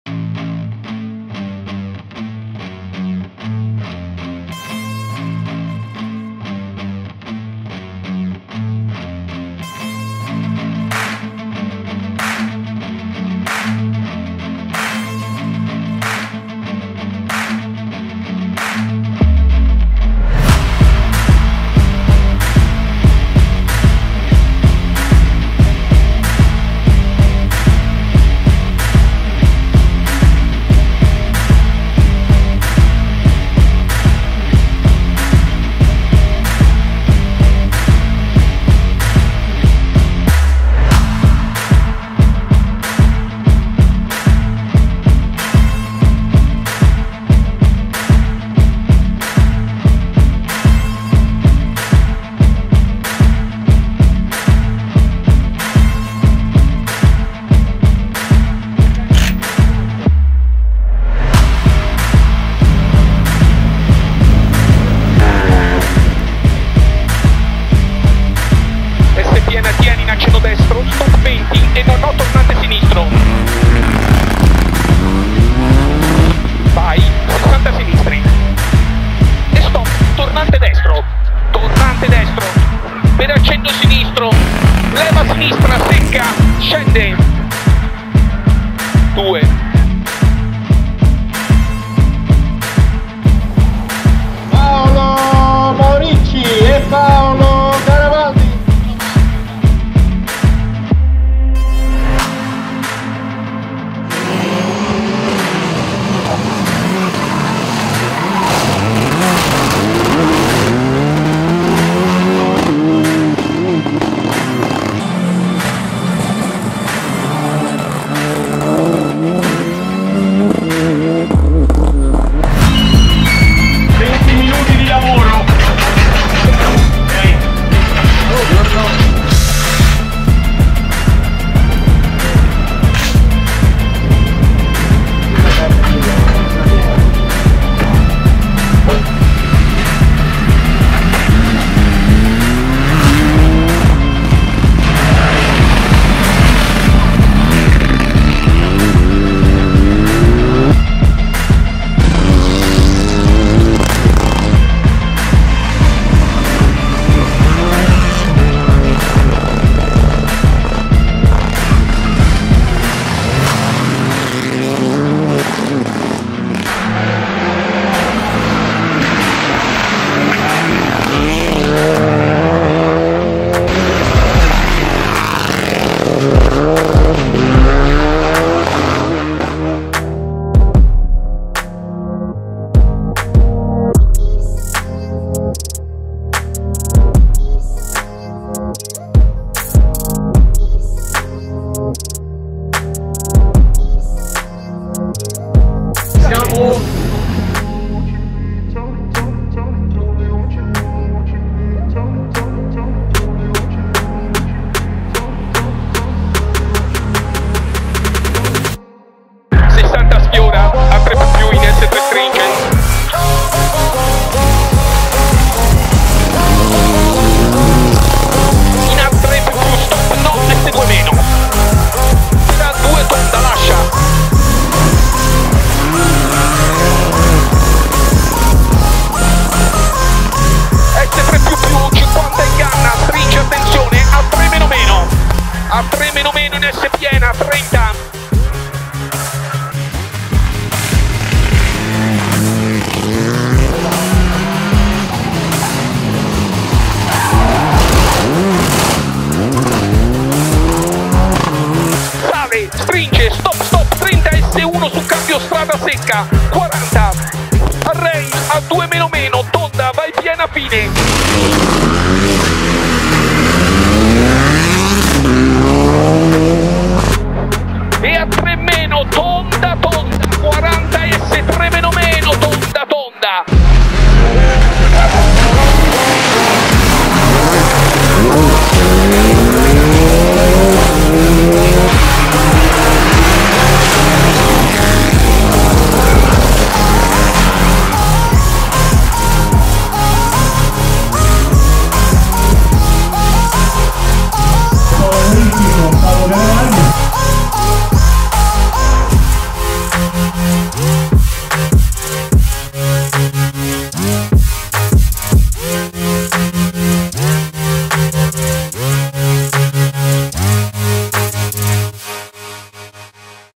Rally del Ciocco 2023 Clip Moricci
Clip riassuntiva della gara dell'equipaggio Moricci-Garavaldi al Rally del Ciocco 2023.